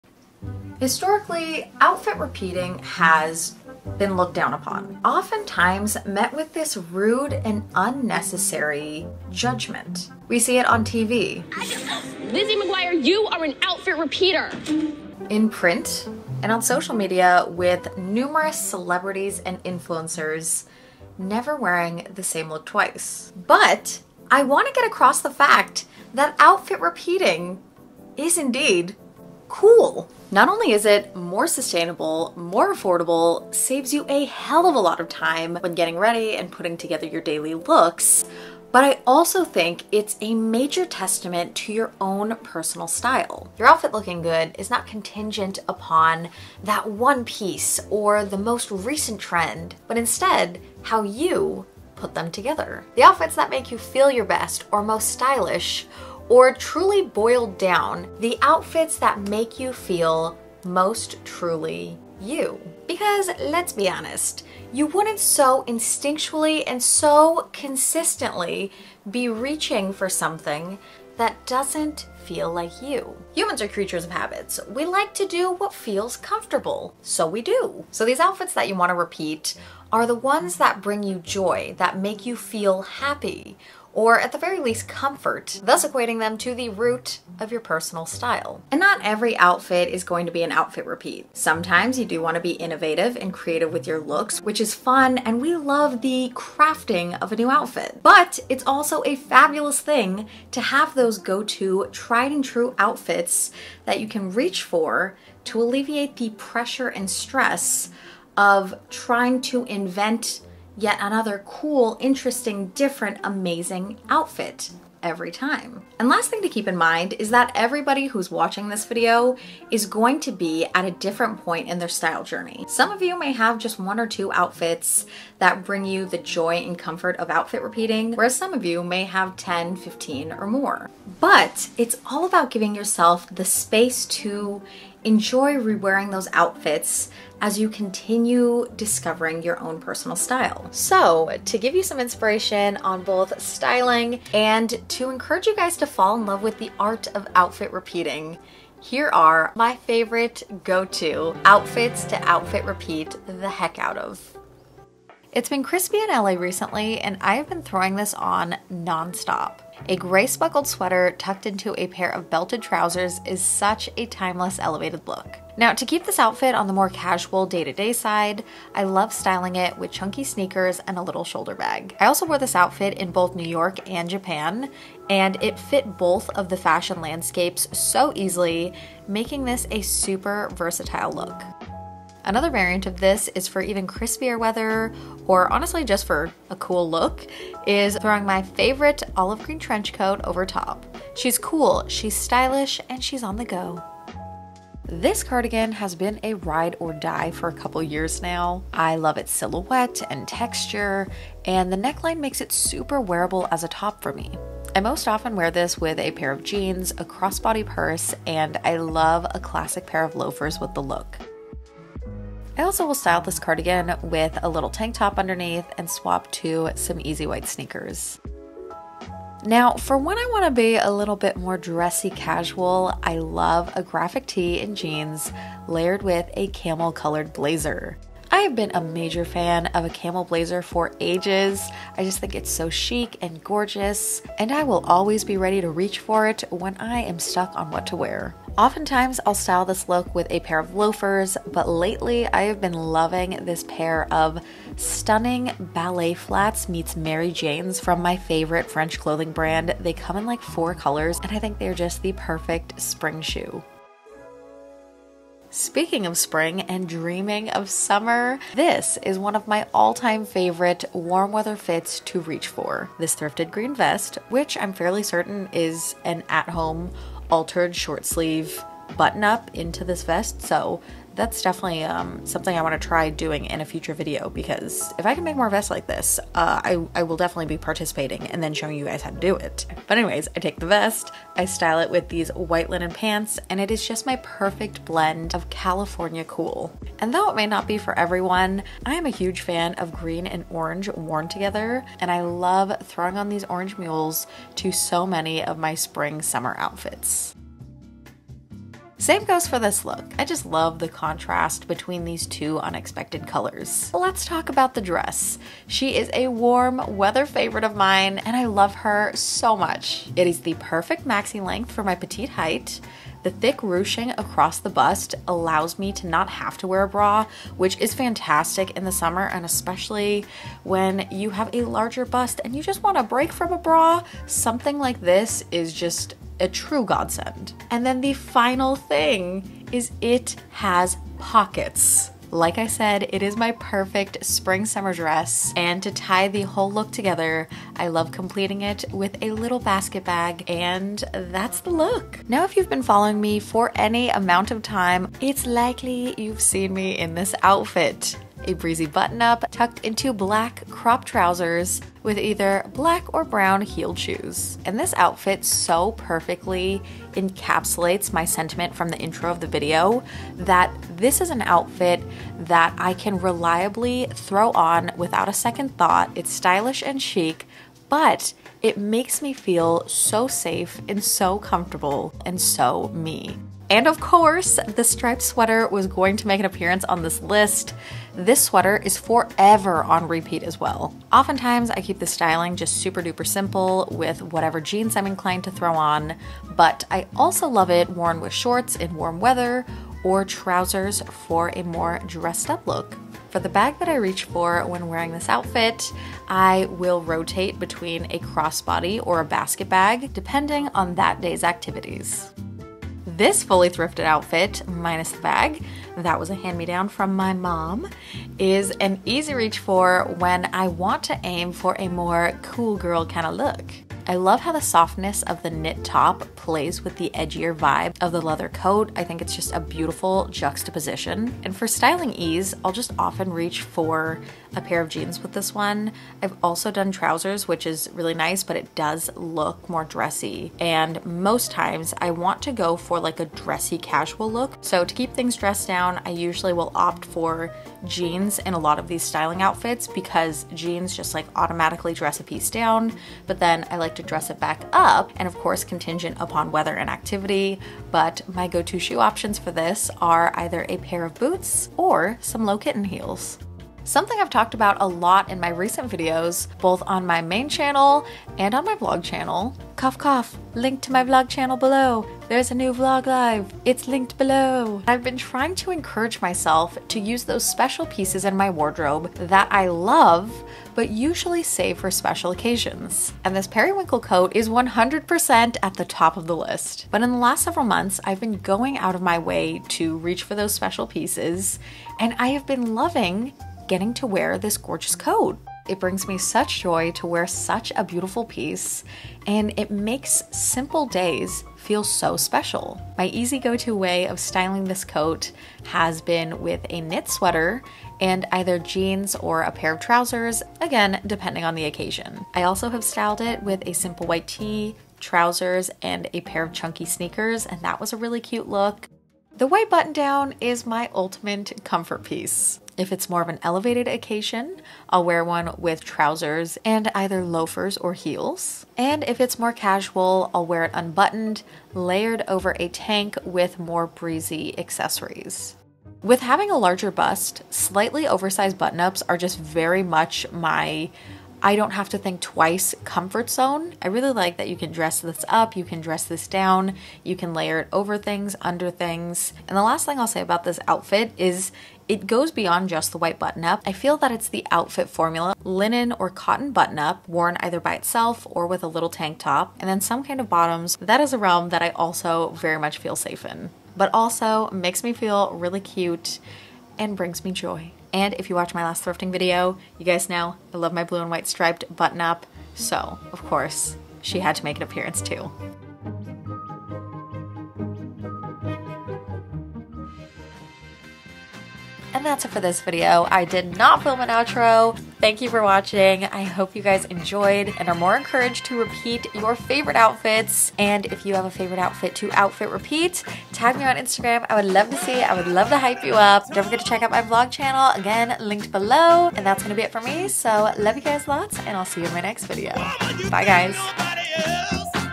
outfit ideas to repeat when you have nothing to wear
Today we're breaking the stigma of outfit repeating! We're chatting about the beauty of rewearing your favorite looks, followed up by me sharing my top favorite looks to style + outfit repeat the heck out of :) newest vlog

Outfit 1
- Grey sweater
- All time fav sneakers (ive had these for a year and wear them 24/7 and they still look this good?! 10/10 recommend)
- Bag (vintage, linking similar)

Outfit 2
- White cardigan (similar)

- Cropped jeans
- Bucket bag (similar) * other similar option
- White sneakers

* another similar option w other colors!

Outfit 4
- Green vest (thrifted, linking similar)
- Linen trousers
- Orange mules (similar)
- Woven bag (thrifted, linking similar)
* another option

Outfit 6
- Button up (similar)
* another similar option
- Trousers (similar)
- Braided belt (thrifted, linking similar)

Outfit 8
- White knit top (thrifted, linking similar)
- Jeans (thrifted, linking similar)
- Leather (thrifted, linking similar)
- Kitten heels (thrifted, linking similar)

Outfit 10
- White button-up
- Black trousers (similar)

Outfit 11
- White button up

Outfit 12
- Blue + white striped (thrifted, linking similar)
- White tank
- White linen pants

jess xx
♡ Links!